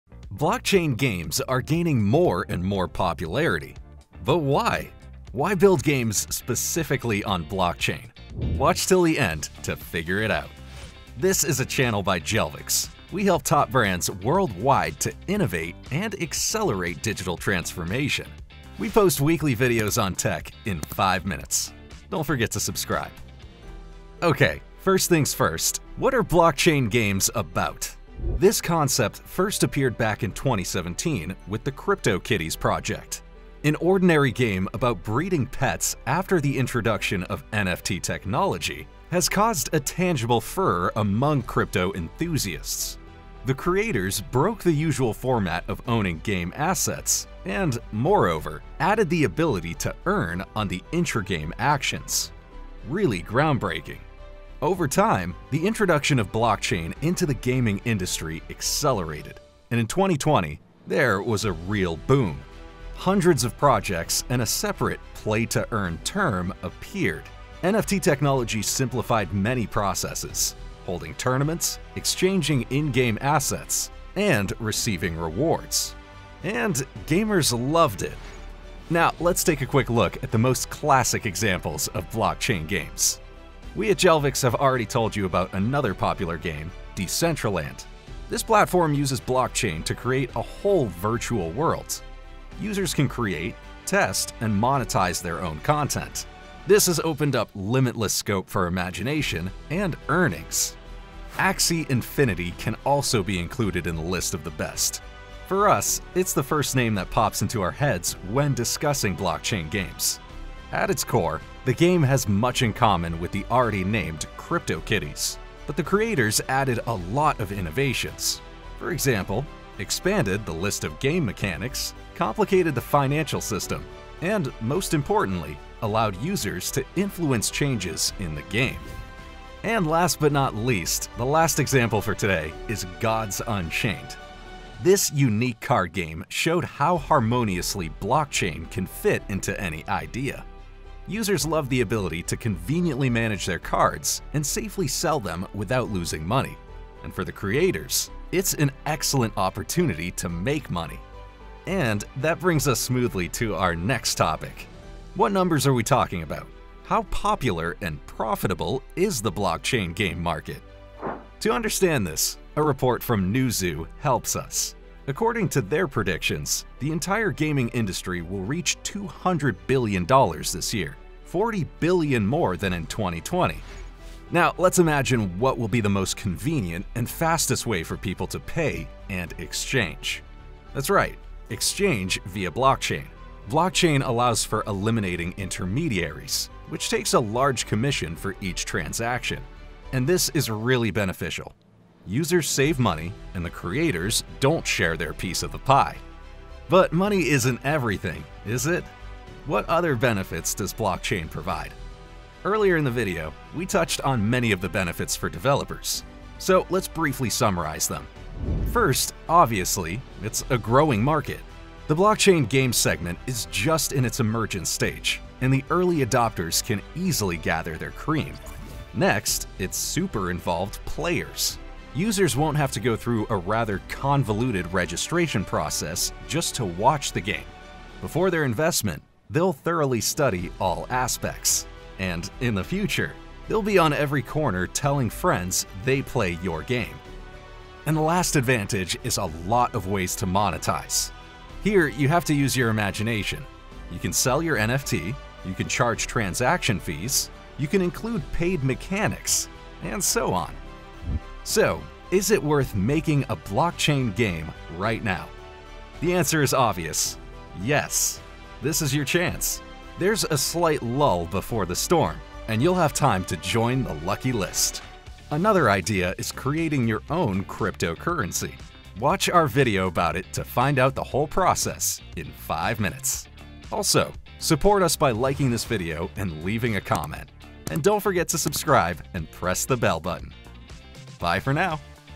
Here's why BLOCKCHAIN games are the future
Blockchain games are gaing more and more popularity, but why? Why build games specifically on blockchain?

▶ Time codes:
00:00 What are blockchain games about?
01:19 Examples of blockchain games
02:35 Are blockchain games even popular?
02:55 Benefits of building games on blockchain
04:25 Watch this next

▶ About this video:
What are blockchain games about?
This concept first appeared back in 2017 with the CryptoKitties project. An ordinary game about breeding pets after the introduction of NFT technology has caused a tangible furor among crypto-enthusiasts. The creators broke the usual format of owning game assets and, moreover, added the ability to earn on their intra-game actions. Really groundbreaking!